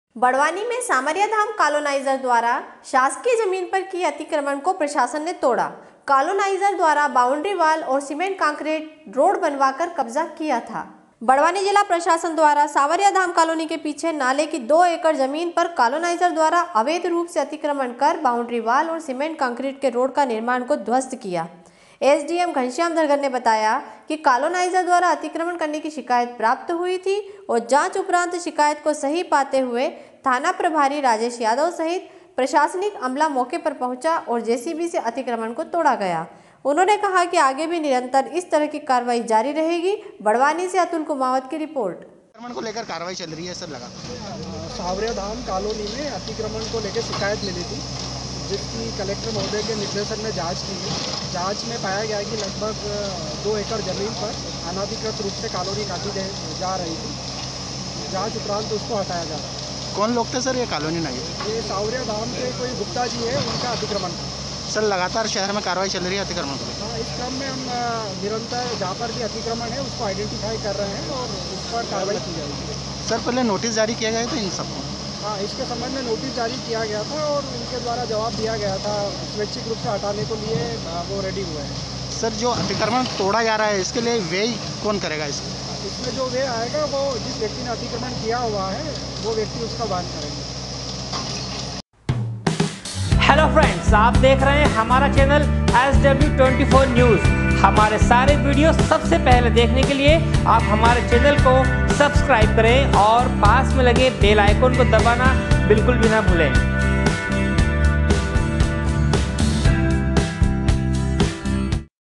Barwani- बड़वानी में सांवरिया धाम कॉलो नाइजर द्वारा शासकीय जमीन पर किए अतिक्रमण को प्रशासन ने तोड़ा।
Barwani- SW 24 News Madhyapradesh barwani SW24news #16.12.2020
बड़वानी में सांवरिया धाम कॉलोनी नाइजर द्वारा शासकीय जमीन पर किए अतिक्रमण को प्रशासन ने तोड़ा। कॉलोनी नाइजर द्वारा बाउंड्रीवाल और सीमेंट कंक्रीट रोड बनाकर कब्जा किया था। बड़वानी से अतुल कुमावत की रिपोर्ट